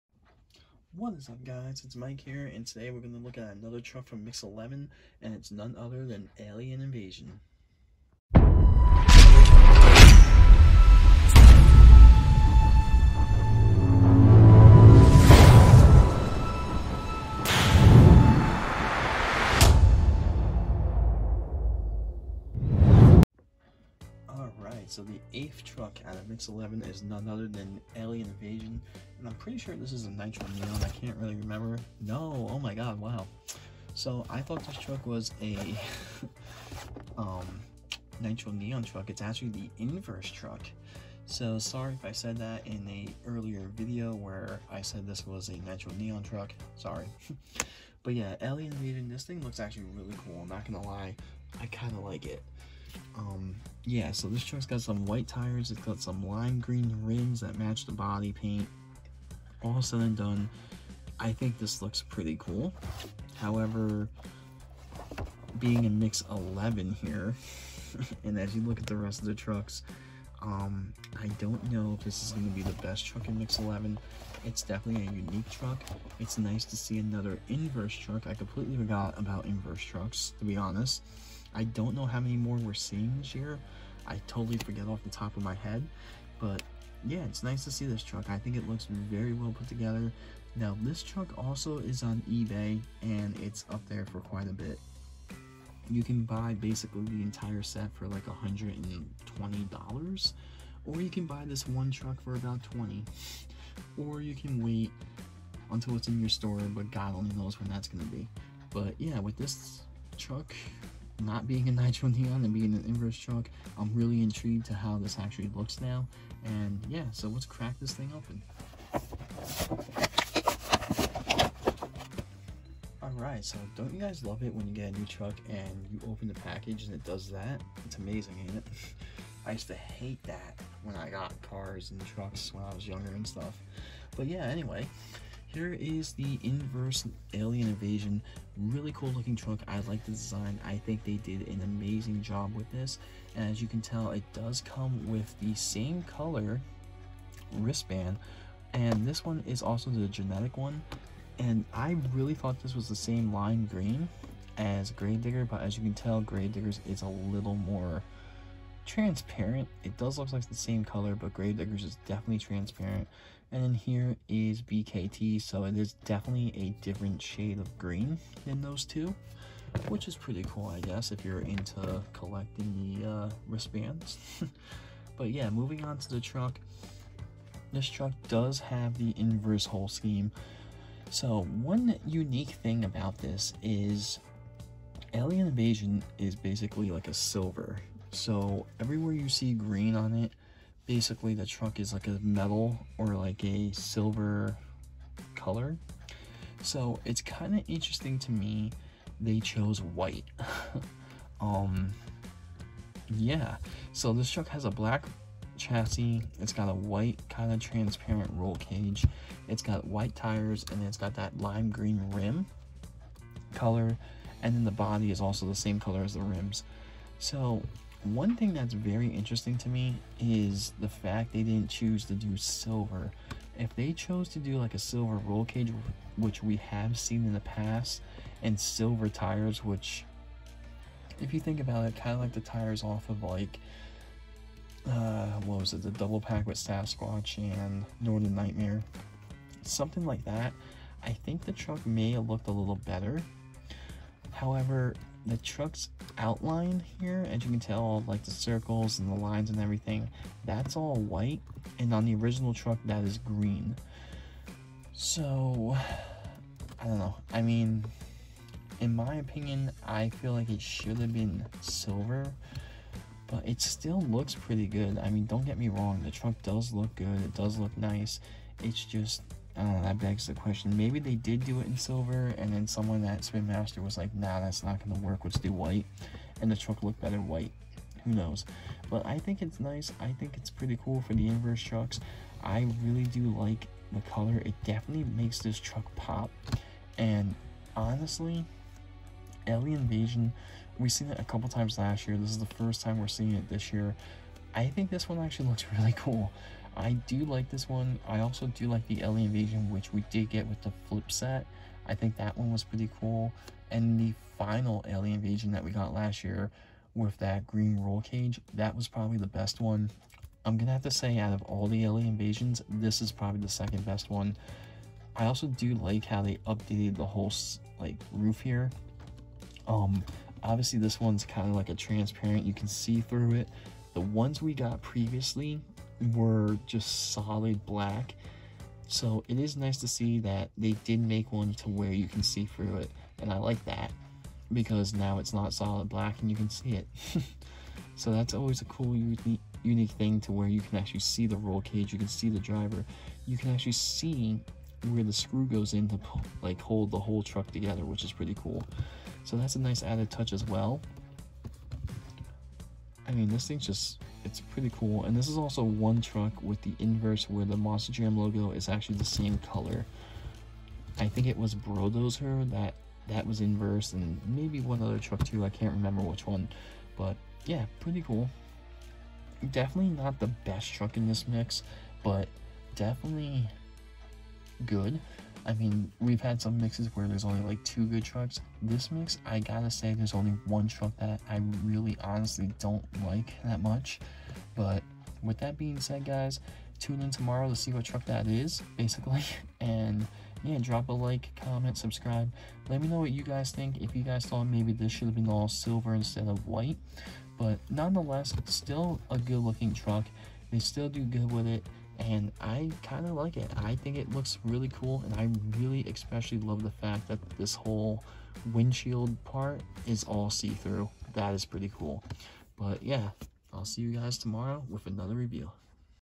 SPIN MASTER MONSTER JAM Mix 11 Alien Invasion Review
Today we look at the 2nd truck out of 3 for the Inverse Trucks For Spin Master's 2020 Monster Jam Toy Line, which is Alien Invasion

Alien Invasion
Mix 11
Lime Green Rims
Pearlescent White Roll Cage
Pearlescent White Tire's
Detailed Decals on Casting/Tooling for 2020
Tinted Windshield and Insert Piece Exclusively for Alien Invasion
Inverse Truck 2/3
Black Chassis